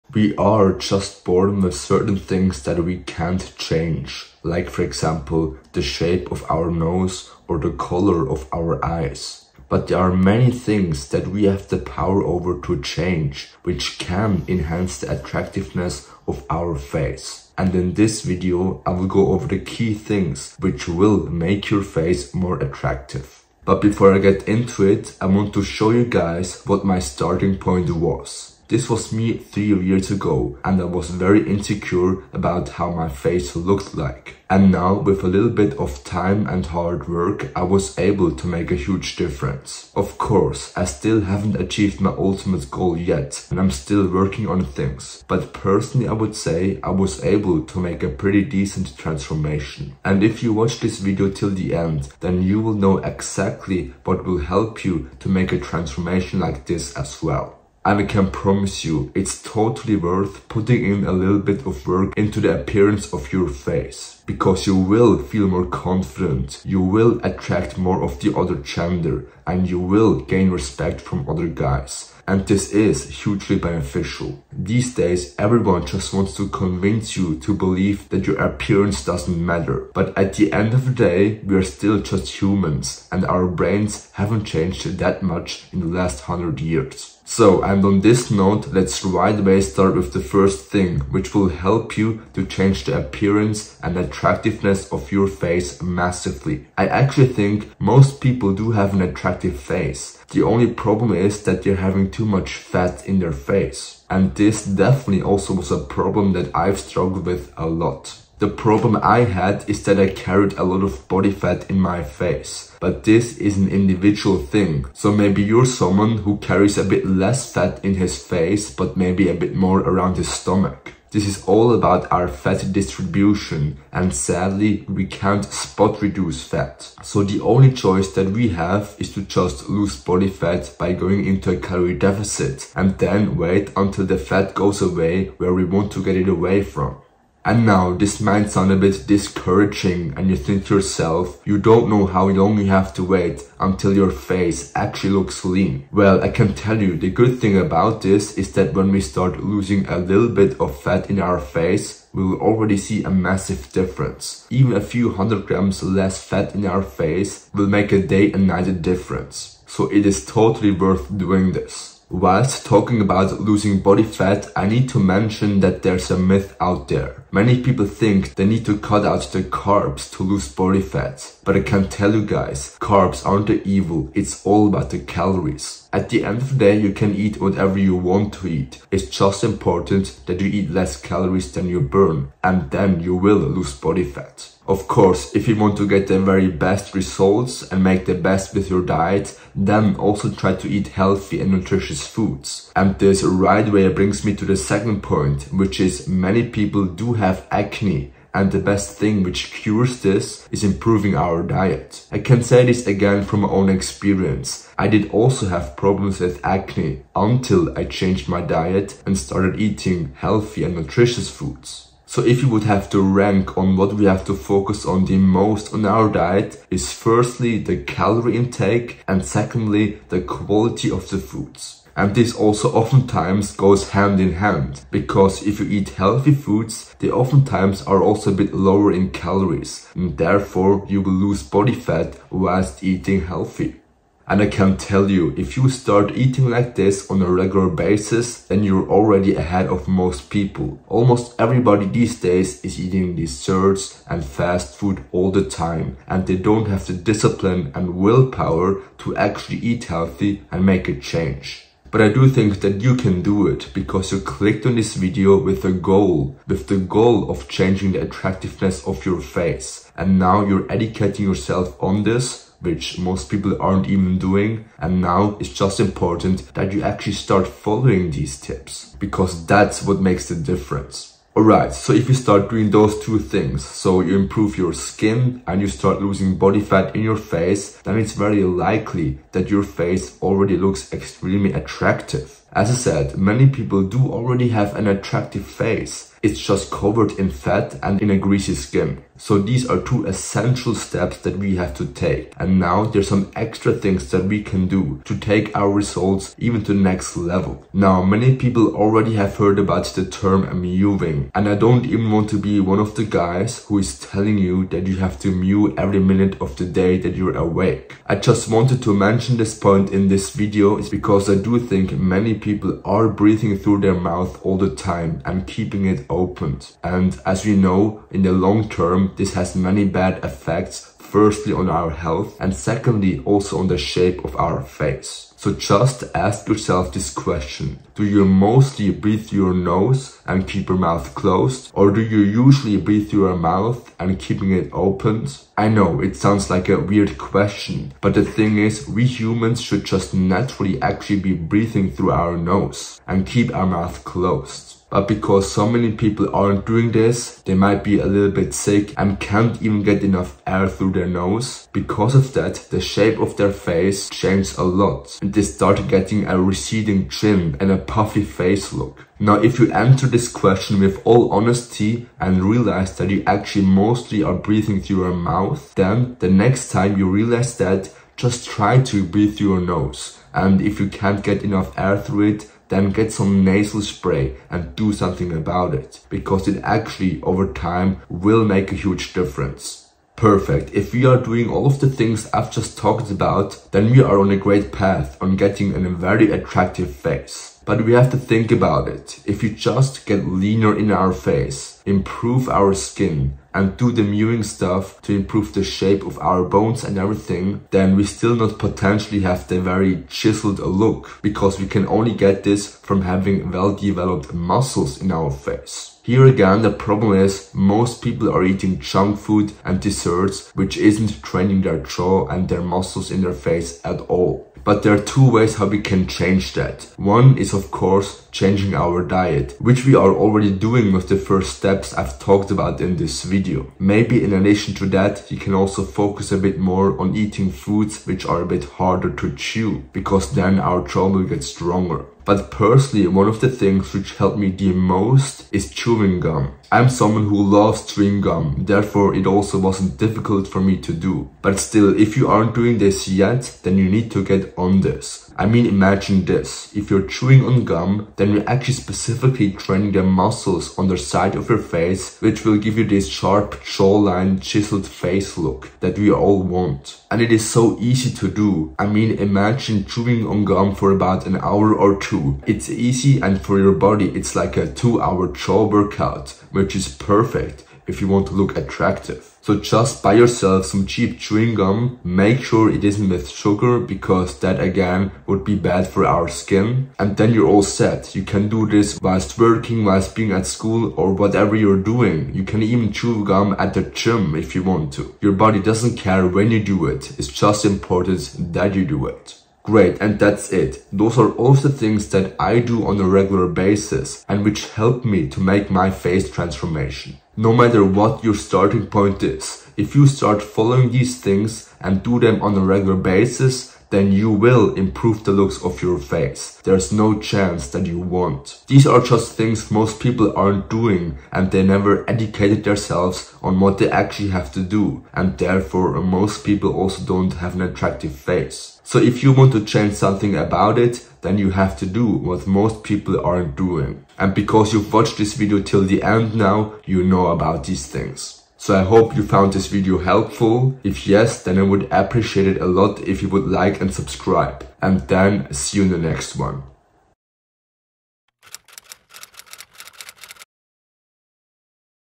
How to Improve Your Facial Attractiveness
These are the things which helped me to get a more attractive face and which will help you as well. If you start doing them now, you'll see progress fast!